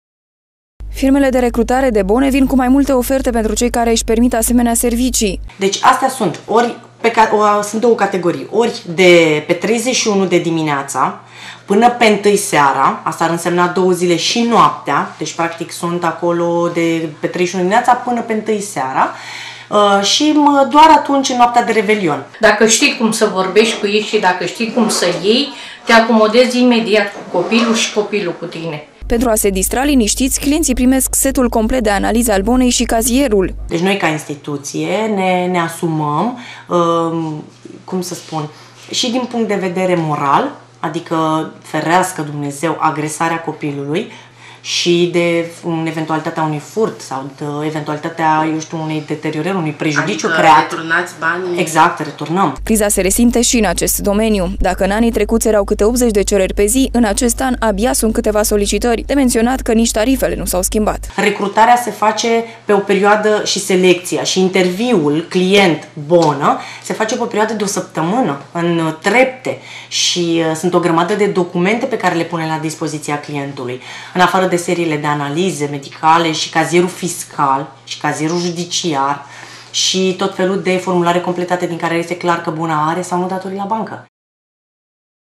Bona de Revelion.mpg
Firmele de recrutare de bone vin cu mai multe oferte pentru cei care isi permit asemenea servicii. Pentru a se distra linistiti clientii primesc setul complet de analiza al bonei si cazierul. Criza se resimte si in acest domeniu, daca in anii trecuti ereau cate 80 de cereri pe zi in acest an abia sunt cateva solicitari. De mentionat ca nici tarifele nu s-au schimbat. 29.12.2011